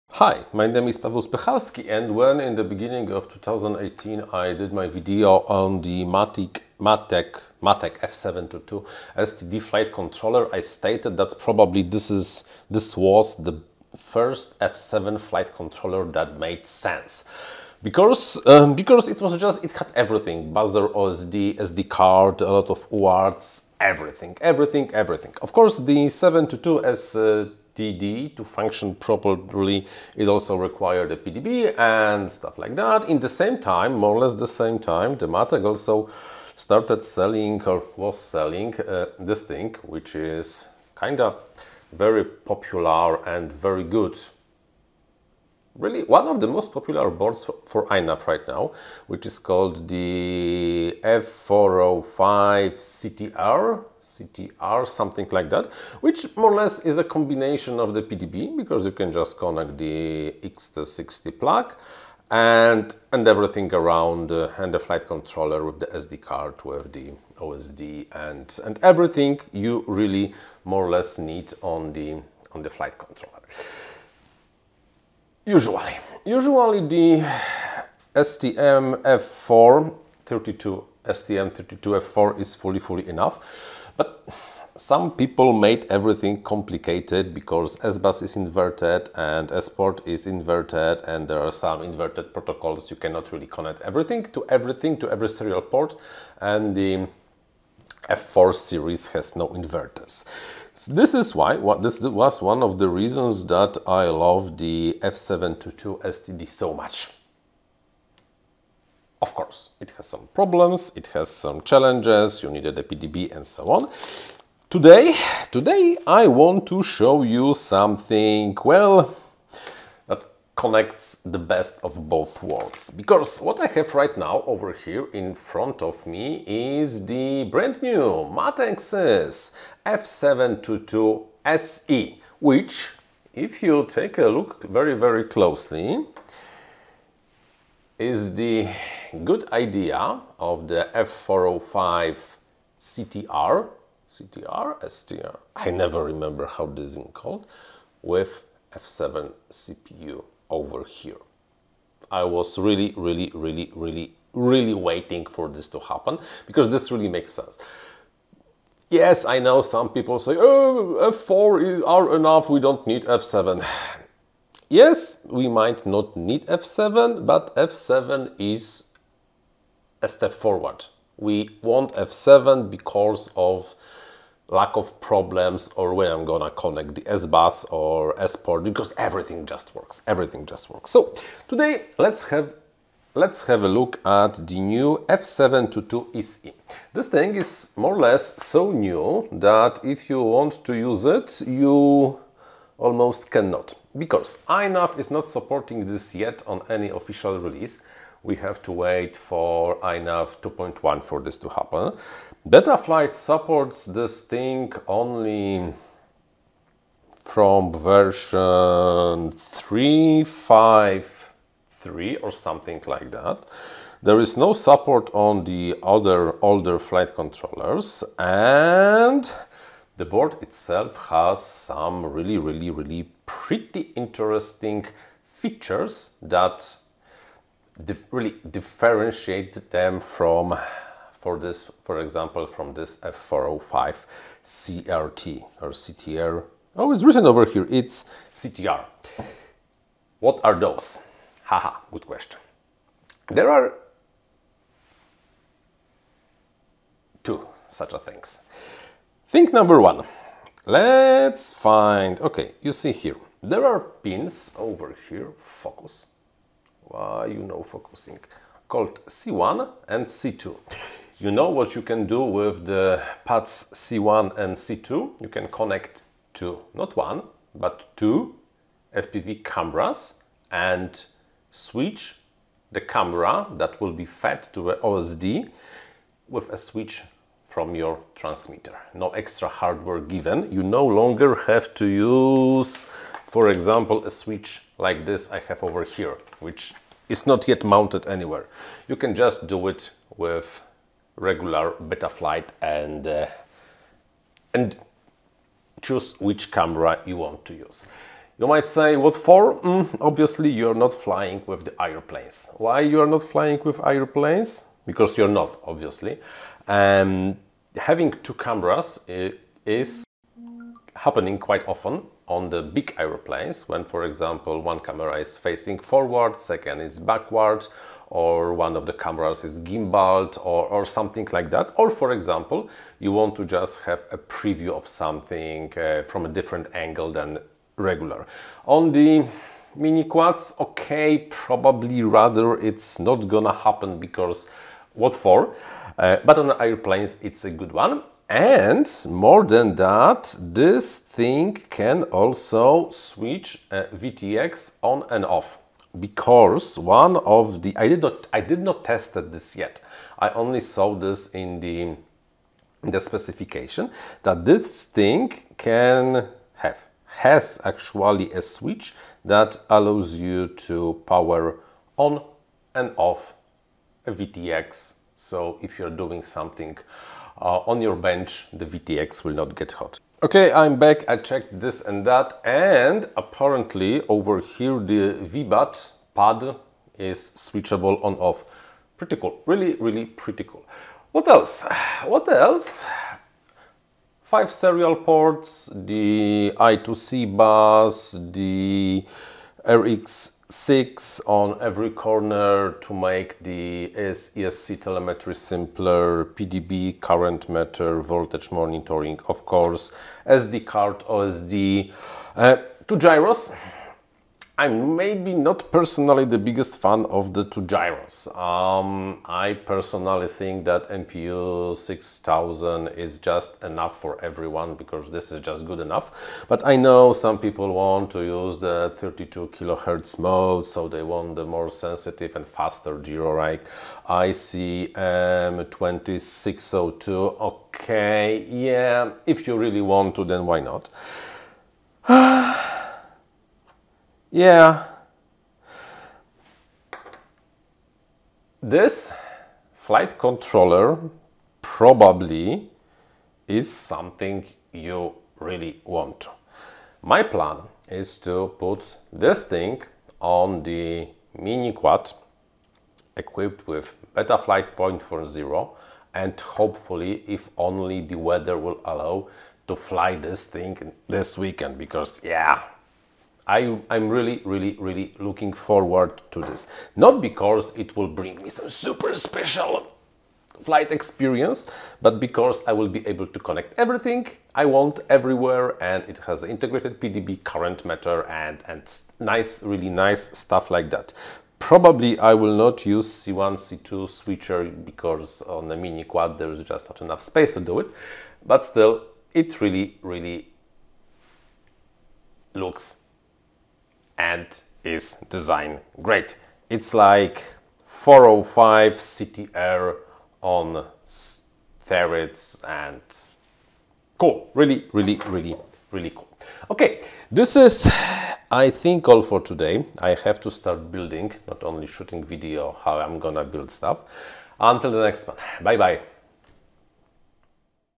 Matek F722-SE - the best F7 flight controller? Maybe...
Say hello to Matek F722-SE , an F7 flight controller with an edge!

* STM32F722 processor
* MPU6000 and ICM20602 user selectable in Betaflight
* OSD compatible with INAV and Betaflight
* BMP280 barometer
* 5 hardware serial ports
* possible 1 software serial
* 8 outputs
* I2C bus
* real PIT mode - Vbat can be turned ON/OFF by the user during flight (or before)
* PDB with a current meter is integrated into the design
* you can connect 2 separate FPV cameras and select the one that will be connected to OSD and VTX with your radio without any additional hardware. Super option for fixed wing flyers
* power VTX on/off from your radio as well (I really will have to test that soon) - Matek F722-SE Vbat pad can be turned ON/OFF
* TX6 is conveniently positioned at every corner for ESC telemetry setup
* F7 means that you can connect anything you want to any serial port you want. F7 series have integrated serial inverters, so SBUS and SmartPort is no longer a problem

Maybe Matek F722-SE is the best flight controller of 2018? Hard to tell, but it is close to that for sure!

Matek F722-SE is coming soon!